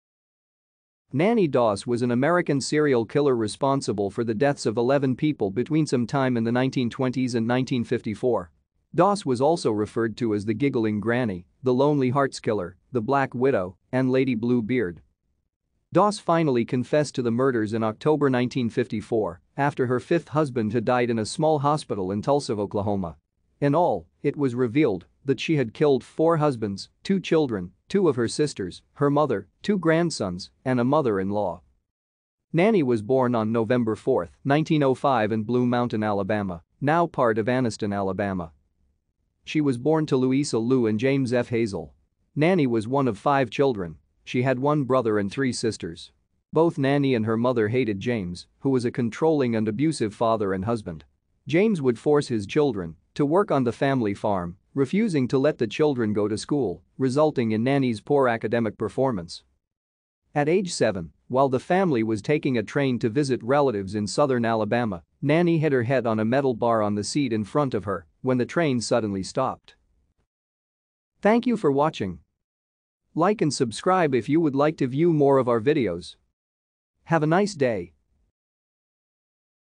Nannie Doss
Nannie Doss  was an American serial killer responsible for the deaths of 11 people between some time in the 1920s and 1954. Doss was also referred to as the Giggling Granny, the Lonely Hearts Killer, the Black Widow, and Lady Blue Beard.